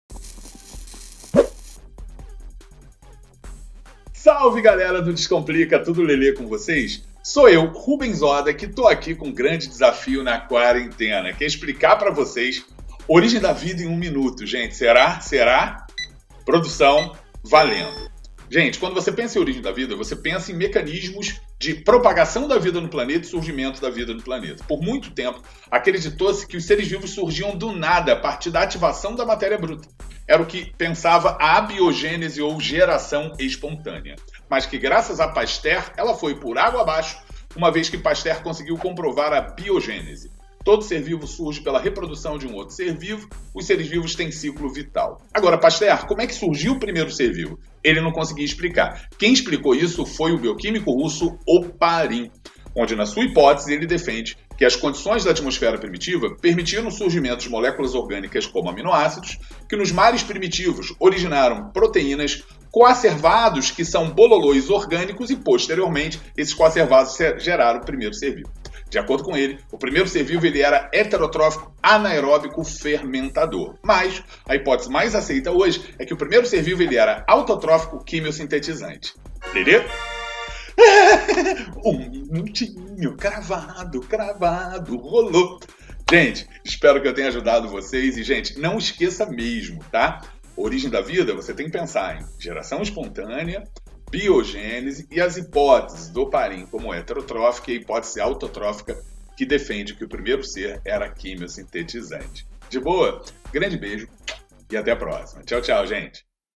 Origem da Vida | Biologia | MINUTO RESUMO
Minuto Resumo na área trazendo um resumo para o Enem de Biologia! O assunto? Origem da vida! É um assunto super comum, cobrado na prova e possível de cair no Enem 2020. Então aproveita esse resumo para o Enem e dá play, porque o vídeo é bem rapidinho! 🦠🌏

Continua com a gente!